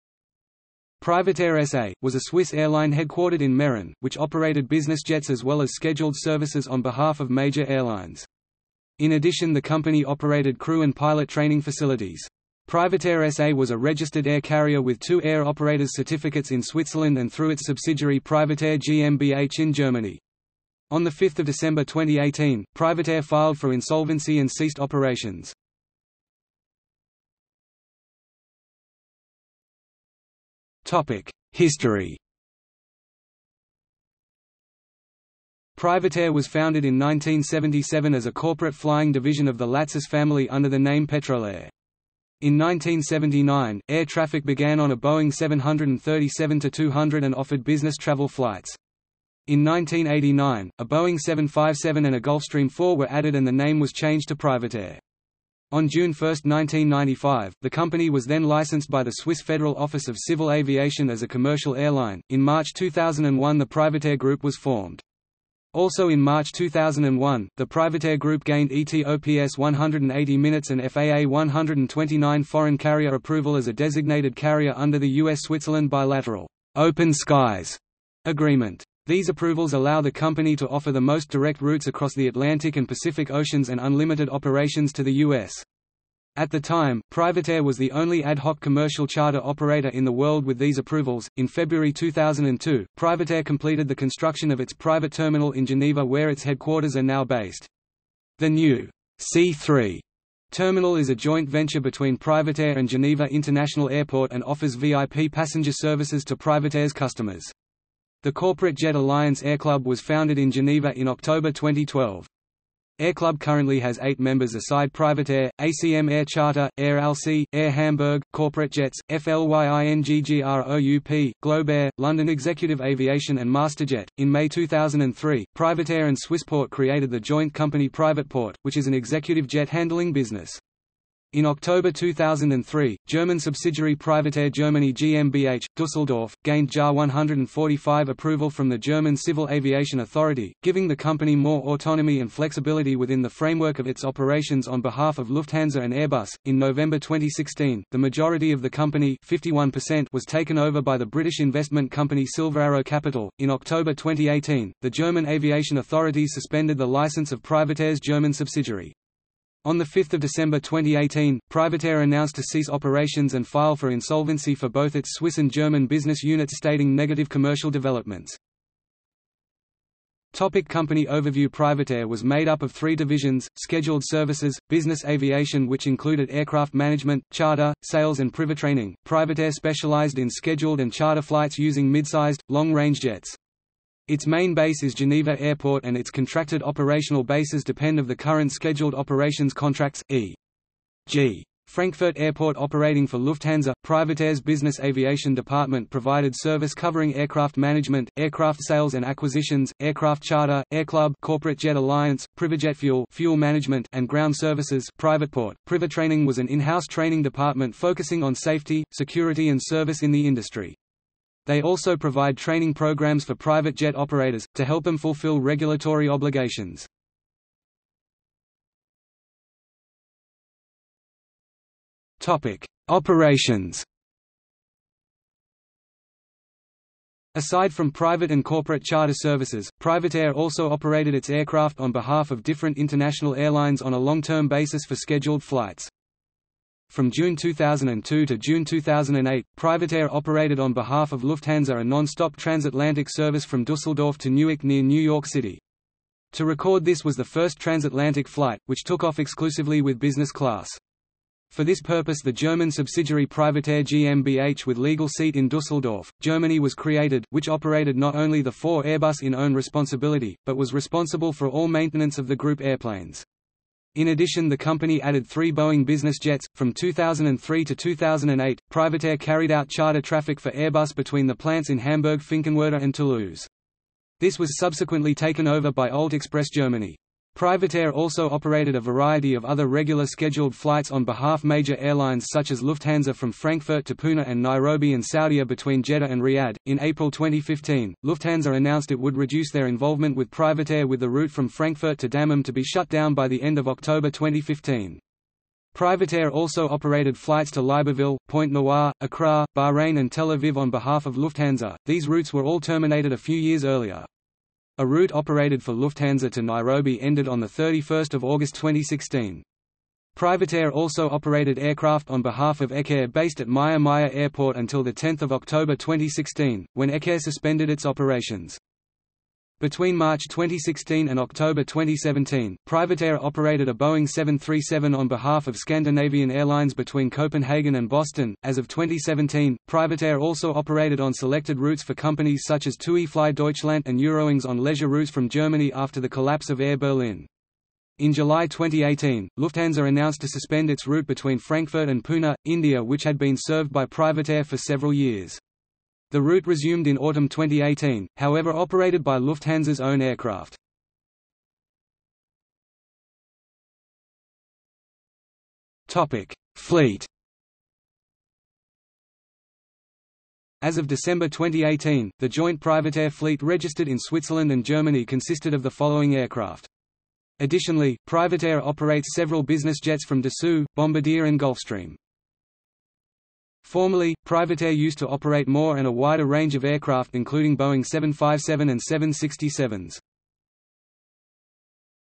PrivatAir | Wikipedia audio article
00:00:27 1 History
00:02:50 2 Company overview
00:03:38 3 Operations
00:05:52 4 Fleet

- Socrates

SUMMARY
Privatair SA, was a Swiss airline headquartered in Meyrin, which operated business jets as well as scheduled services on behalf of major airlines. In addition the company operated crew and pilot training facilities. PrivatAir SA was a registered air carrier with two air operator's certificates in Switzerland and through its subsidiary PrivatAir GmbH in Germany. On 5 December 2018, PrivatAir filed for insolvency and ceased operations.